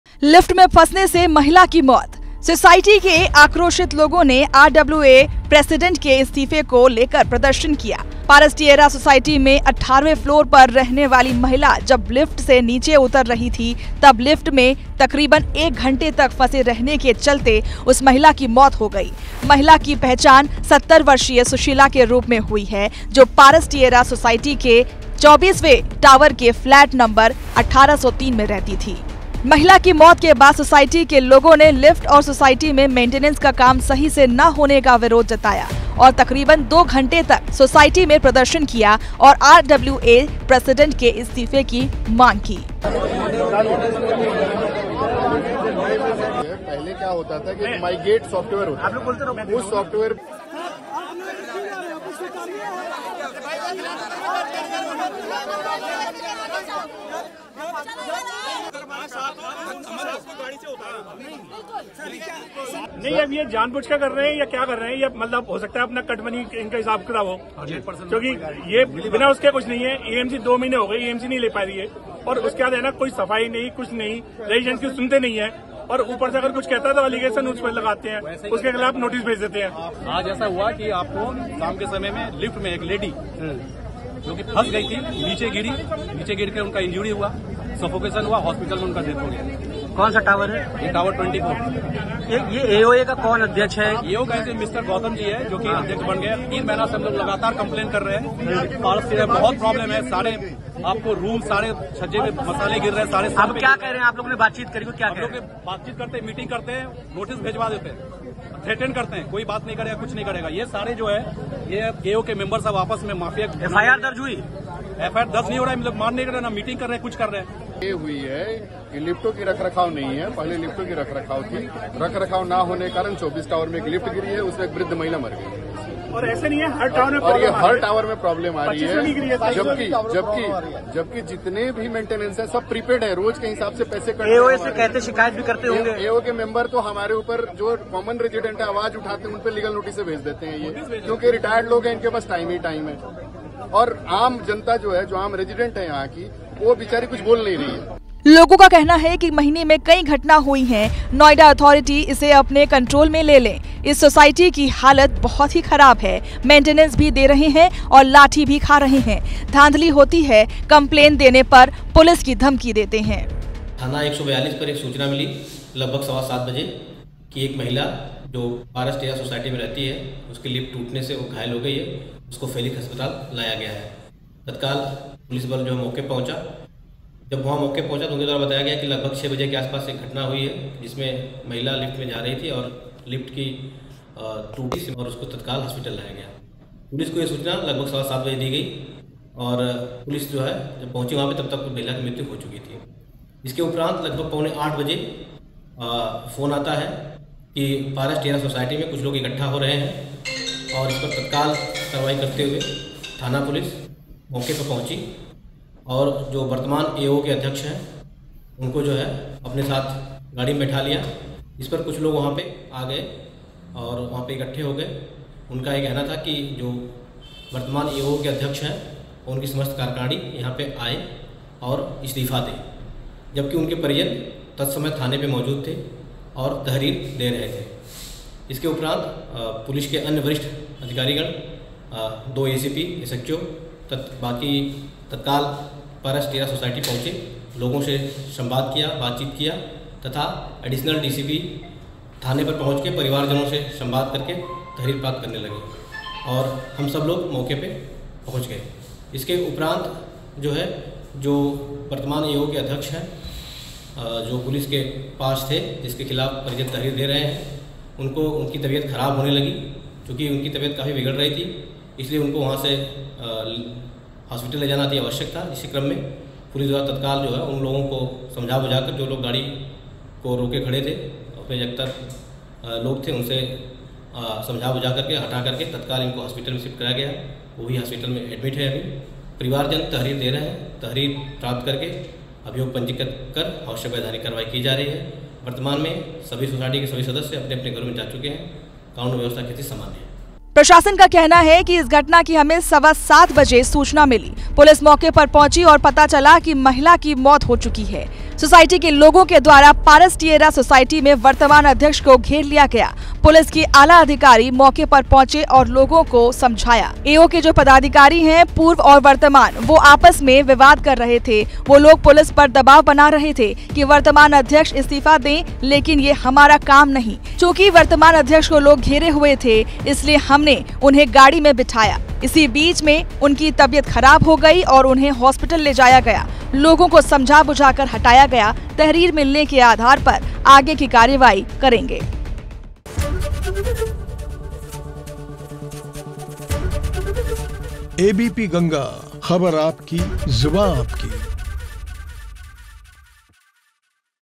UP NEWS : 8वें फ्लोर से गिरी लिफ्ट तड़प तड़प कर तोड़ा दम
लिफ्ट में फंसने से महिला की मौत, सोसाइटी के आक्रोशित लोगों ने RWA प्रेसिडेंट के इस्तीफे को लेकर प्रदर्शन किया.पारस टियरा सोसाइटी में 18वें फ्लोर पर रहने वाली महिला जब लिफ्ट से नीचे उतर रही थी तब लिफ्ट में तकरीबन 1 घंटे तक फंसे रहने के चलते उस महिला की मौत हो गयी. महिला की पहचान 70 वर्षीय सुशीला के रूप में हुई है जो पारस टियरा सोसाइटी के 24 वें टावर के फ्लैट नंबर 1803 में रहती थी.